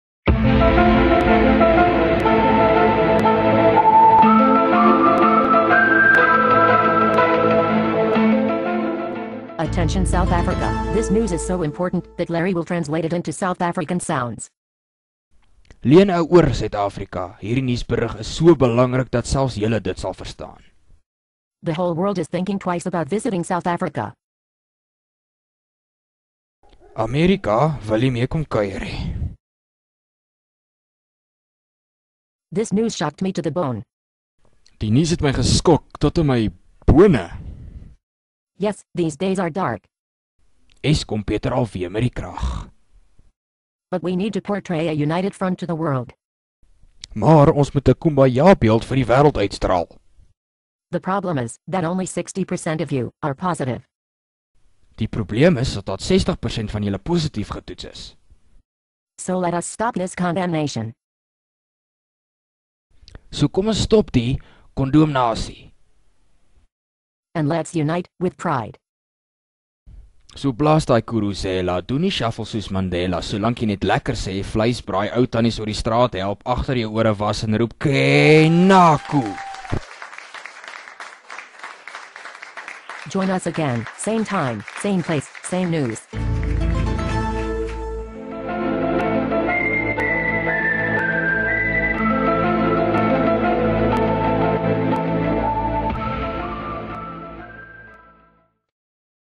South Africa - Lost in Translation
With the current political turmoil in South Africa and the FIFA 2010 World Cup less than 60 days away, this humerous piece shows how words can be twisted and misintepreted and pleads with South Africans to stop its childish bickering and unite.